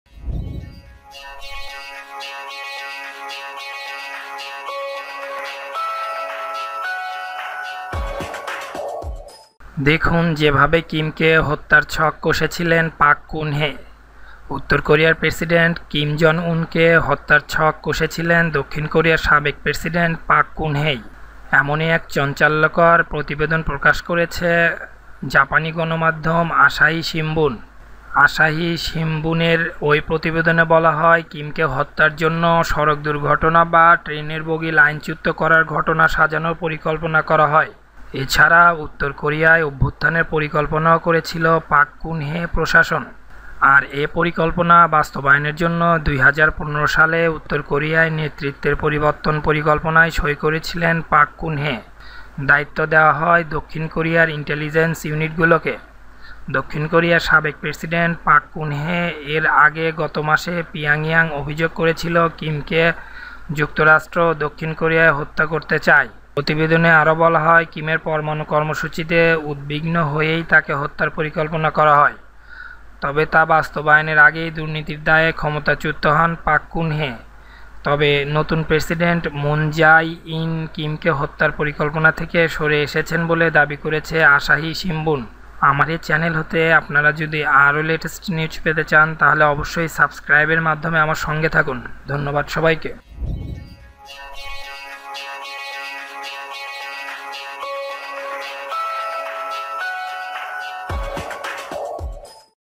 দেখুন যে ভাবে কিমকে হত্যার ছক কষে ছিলেন পাক কুন হে!
উত্তর কোরিয়ার প্রেসিডেন্ট কিম জং-উনকে হত্যার ছক কষে ছিলেন দক্ষিণ কোরিয়ার সাবেক প্রেসিডেন্ট পাক কুন হে। এমনই এক চাঞ্চল্যকর প্রতিবেদন প্রকাশ করেছে জাপানি গণমাধ্যম আশাহি শিম্বুন।

আশাহি শিম্বুনের ওই প্রতিবেদনে বলা হয়,  কিমকে হত্যার জন্য সড়ক দুর্ঘটনা বা ট্রেনের বগি লাইনচ্যুত করার ঘটনা সাজানোর পরিকল্পনা করা হয়। এছাড়া, উত্তর কোরিয়ায় অভ্যুত্থানের পরিকল্পনাও করেছিল পাক কুন হে প্রশাসন।

আর এ পরিকল্পনা বাস্তবায়নের জন্য ২০১৫ সালে উত্তর কোরিয়ায় ‘নেতৃত্বের পরিবর্তন’ পরিকল্পনায় সই করেন পাক কুন হে। দায়িত্ব দেয়া হয় দক্ষিণ কোরিয়ার ইন্টিলিজেন্স ইউনিটগুলোকে।

দক্ষিণ কোরিয়ার সাবেক প্রেসিডেন্ট পাক কুন হে

এর আগে গতমাসে পিয়ংইয়ং অভিযোগ করেছিল কিম কে যুক্তরাষ্ট্র ও দক্ষিণ কোরিয়া হত্যা করতে চায়।

প্রতিবেদনে আরো বলা হয়, কিমের পরমাণু কর্মসূচিতে উদ্বিগ্ন হয়েই তাকে হত্যার পরিকল্পনা করা হয়। তবে তা বাস্তবায়েনর আগেই দুর্নীতির দায়ে ক্ষমতাচ্যুত হন পাক কুন হে। তবে নতুন প্রেসিডেন্ট মুন-জাই- ইন কিমকে হত্যার পরিকল্পনা থেকে সরে এসেছেন বলে দাবি করেছে আশাহি শিম্বুন।